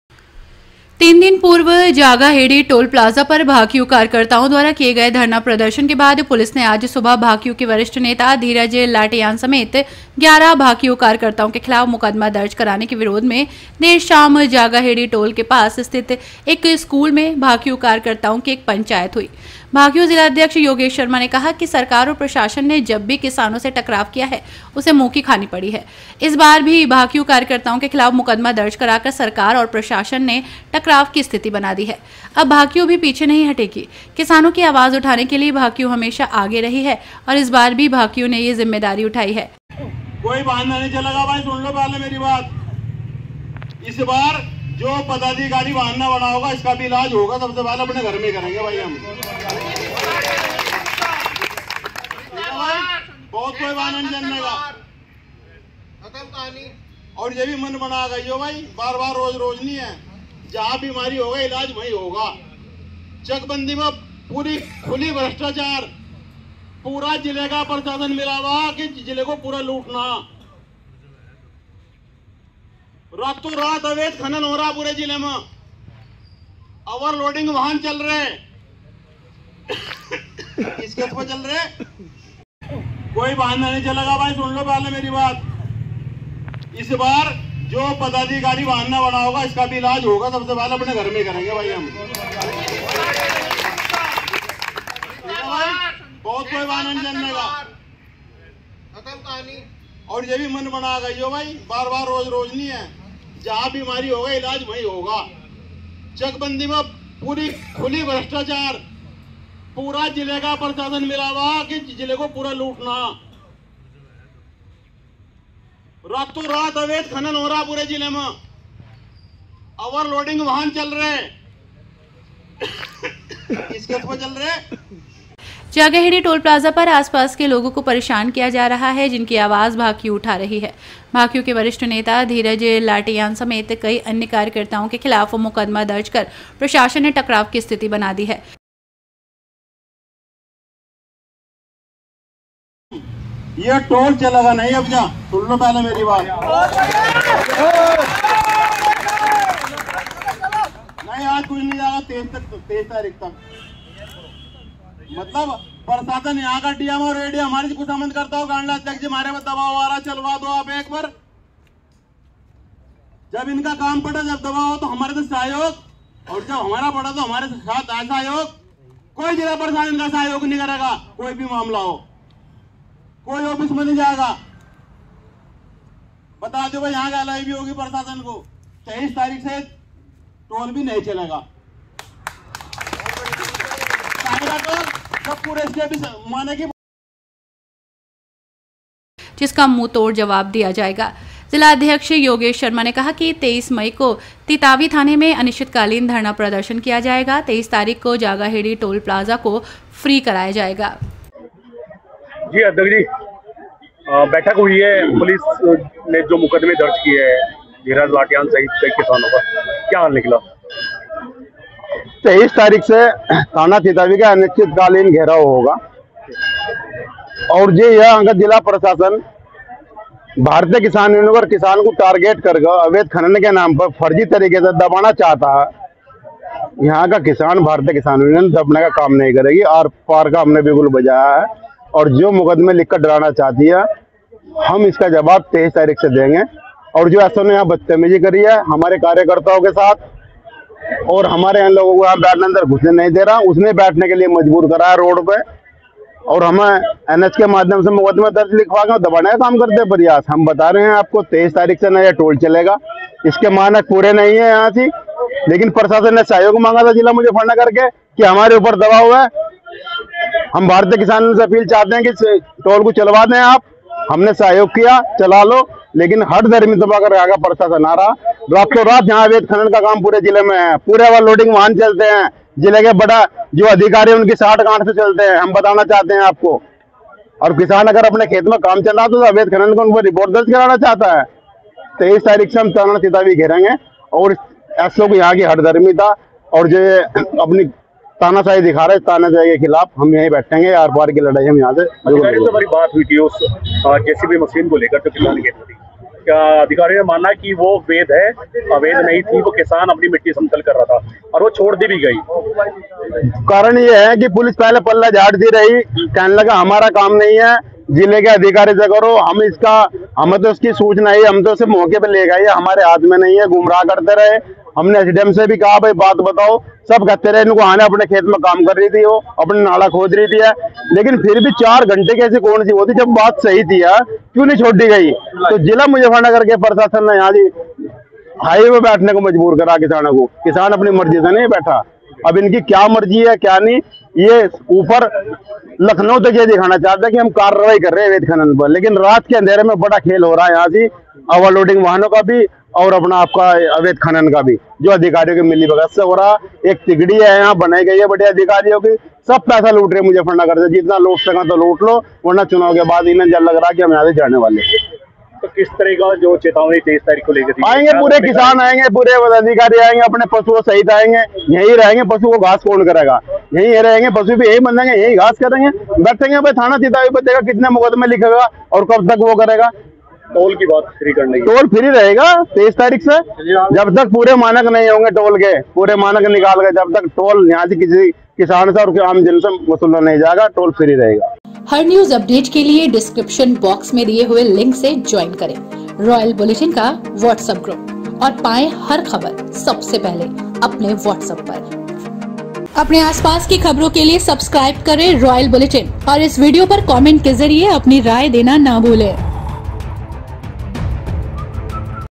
मुज़फ्फरनगर में भाकियू की प्रशासन से फिर ठनी, 23 से धरना शुरू, टोल भी बंद कराएंगे
मुजफ्फरनगर। तीन दिन पूर्व जागाहेडी टोल प्लाजा पर भाकियू कार्यकर्ताओं द्वारा किए गए धरना प्रदर्शन के बाद पुलिस द्वारा आज सुबह भाकियू के वरिष्ठ नेता धीरज लाटियान समेत 11 भाकियू कार्यकर्ताओं के खिलाफ मुकदमा दर्ज कराने के विरोध में आज देर शाम जागाहेडी टोल के पास स्थित एक स्कूल में भाकियू कार्यकर्ताओं की एक पंचायत हुई, जिसमें भाकियू जिलाध्यक्ष योगेश शर्मा ने कहा कि सरकार व प्रशासन ने जब भी किसानों से टकराव किया है, उसे मुंह की खानी पड़ी है।

 इस बार भी भाकियू कार्यकर्ताओं के खिलाफ मुकदमा दर्ज कराकर सरकार व प्रशासन ने टकराव की स्थिति बना दी है, अब भाकियू भी पीछे नहीं हटेगकिसानों की आवाज उठाने के लिए भाकियू हमेशा आगे रही है और इस बार भी भाकियू ने यह जिम्मेदारी उठाई है। जागाहेडी टोल प्लाजा पर आसपास के लोगों को परेशान किया जा रहा है, जिनकी आवाज भाकियू उठा रही है, भाकियू के वरिष्ठ नेता धीरज लाटियान समेत कई अन्य कार्यकर्ताओं के खिलाफ मुकदमा दर्ज कर प्रशासन ने टकराव की स्थिति बना दी है, जिसका मुंहतोड़ जवाब दिया जाएगा।

भाकियू जिलाध्यक्ष योगेश शर्मा ने कहा कि आगामी 23 मई को तितावी थाने में अनिश्चितकालीन धरना प्रदर्शन किया जाएगा और 23 तारीख को जागाहेड़ी टोल प्लाजा को फ्री कराया जाएगा।